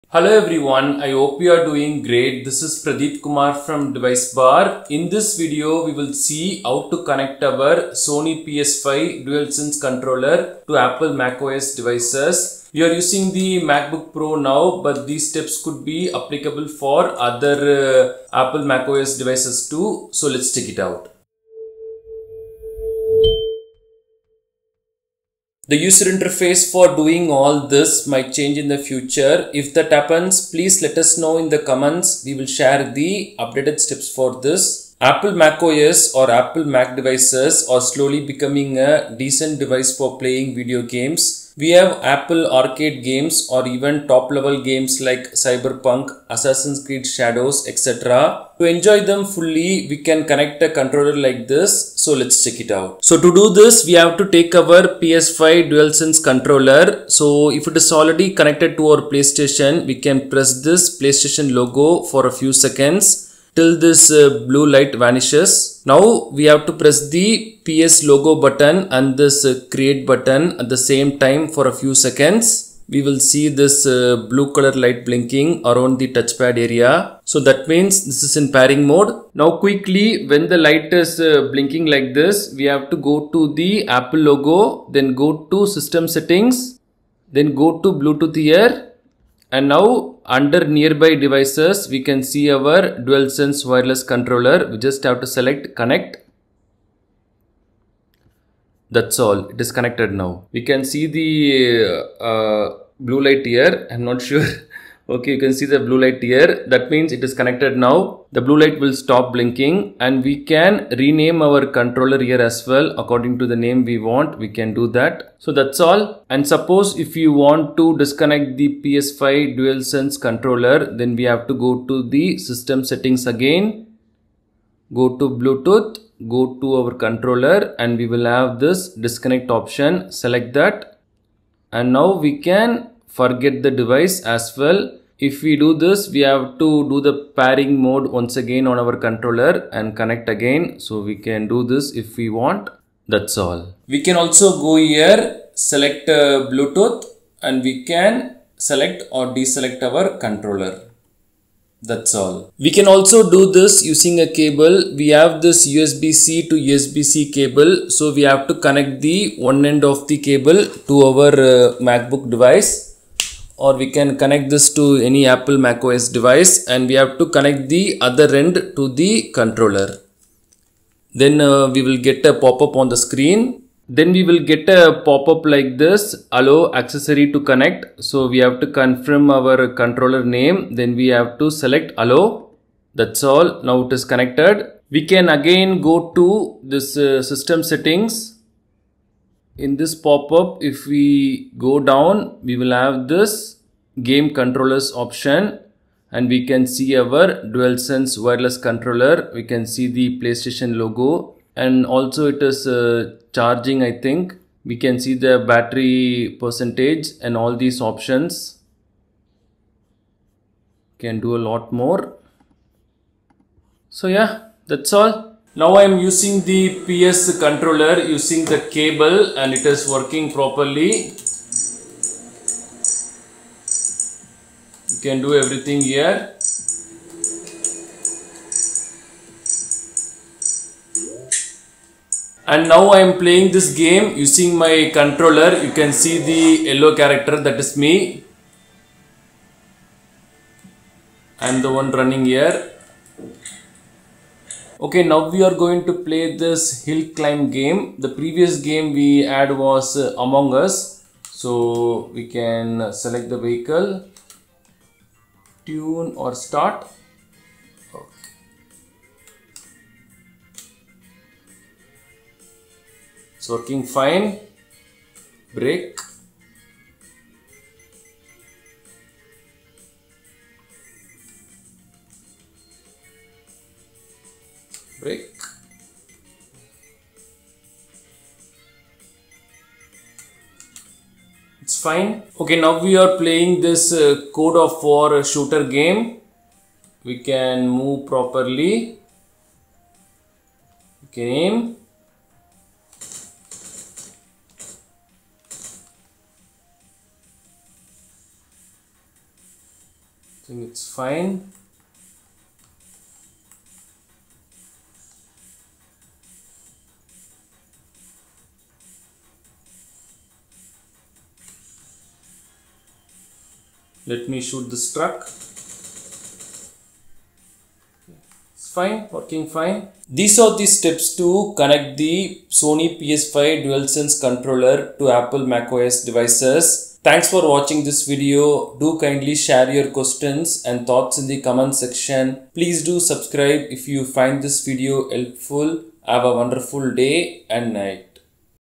How To Connect Sony PS5 DualSense Controller To Apple macOS Devices! (2 Ways)
Steps to pair Sony PlayStation 5 DualSense controller to Apple macOS devices like MacBook.